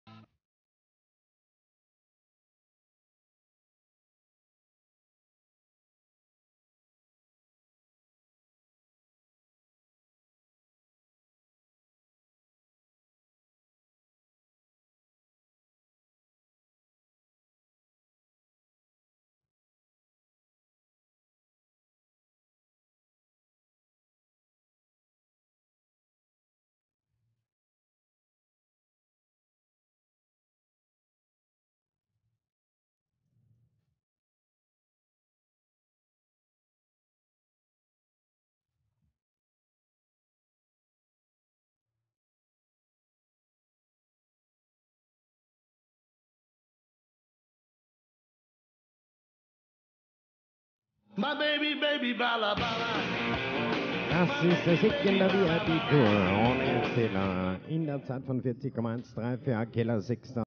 Sieger Final VIP Tour - Sixta Akela mit Ab 19 Gipsy King...
CSI2* 13. - 16. Februar 2014 MAGNA RACINO WINTER TOUR 2014 - WEEK 1
Alle Ritte zum Analysieren und Bewundern in der MAGNA RACINO Mediathek